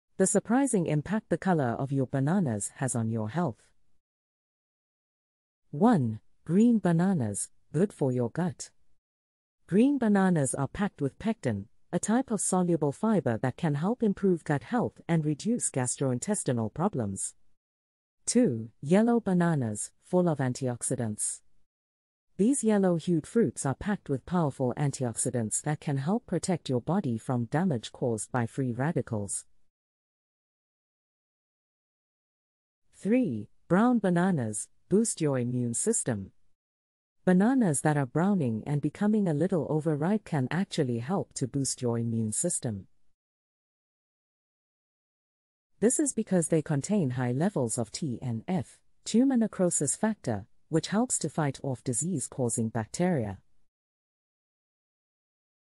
The Surprising Impact The Color of Your Bananas Has on Your Health
Have you ever noticed the color of your banana when you buy it? Did you know that the color of the banana you eat can have an impact on your health? Most of us are used to buying yellow bananas, but they can come in a variety of colors, from green to red. In this video, we will explore the surprising impact the color of your banana has on your health, and the benefits of eating different colored bananas. You may be surprised by what you learn!